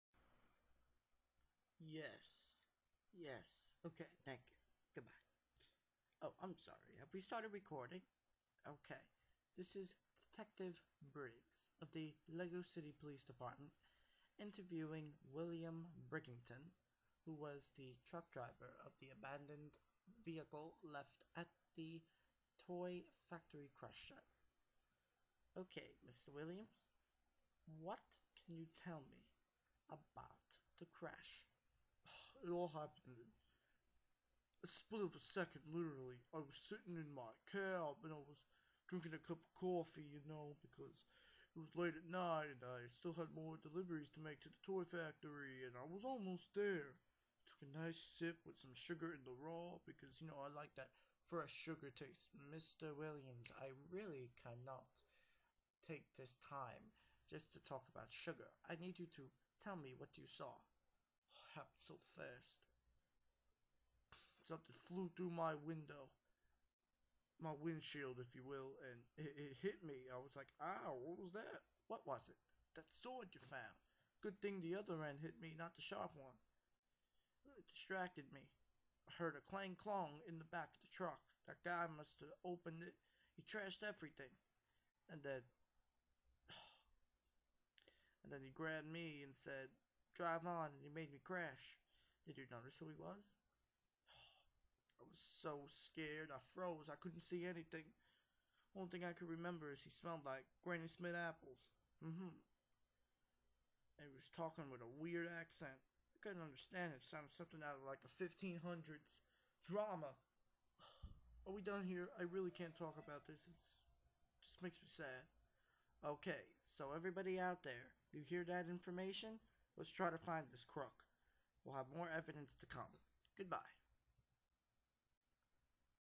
LEGO City Police Evidence File #4
WHO DID IT? CAN THIS SCARED TRUCK DRIVER'S STATEMENT BE ENOUGH FOR THE LEGO CITY POLICE DEPARTMENT TO FIND OUT WHO STOLE THE TOYS? FIND OUT!